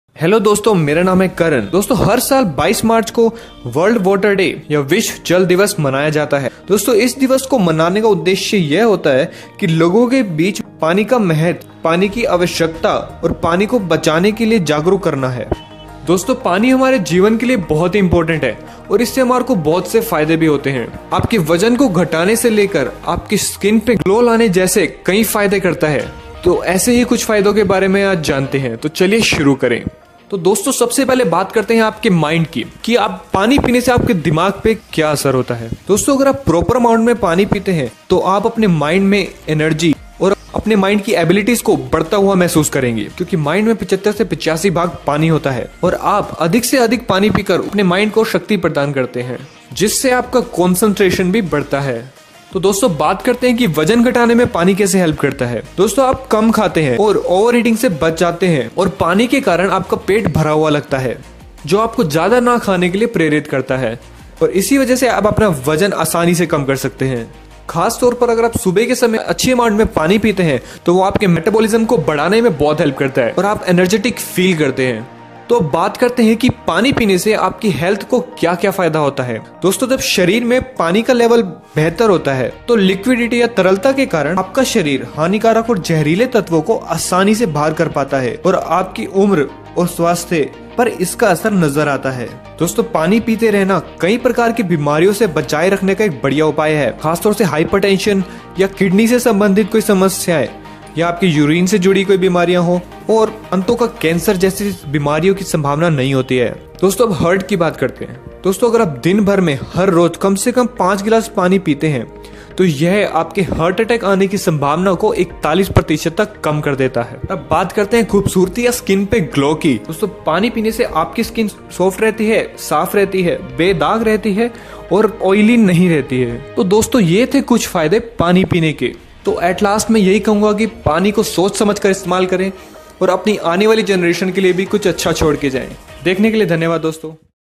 पानी पीने के फायदे। वजन घटाये पानी पी कर Amazing Benefits of drinking water
Amazing Benefits of Drinking Water - Lose Weight By Drinking Water - Hindi